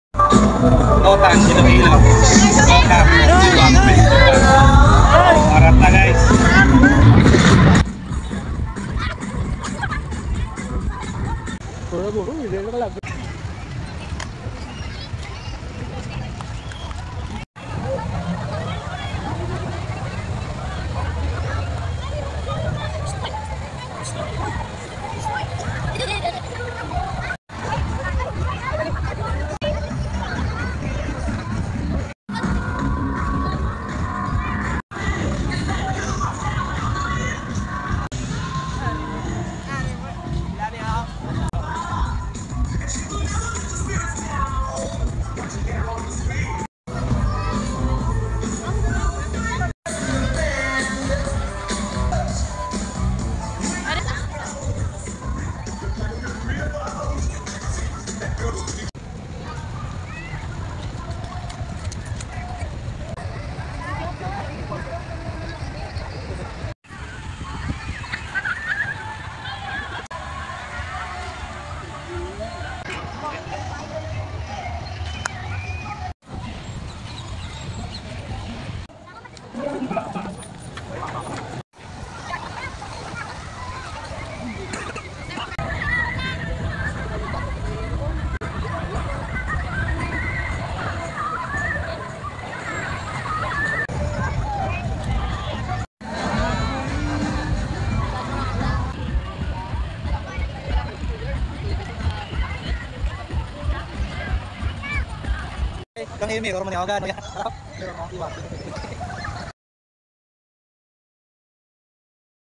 IWAG 2023Ang ganda ng mga lights dito sa Pototan, Iloilo Public Plaza
IWAG 2023

Ang ganda ng mga lights dito sa Pototan, Iloilo Public Plaza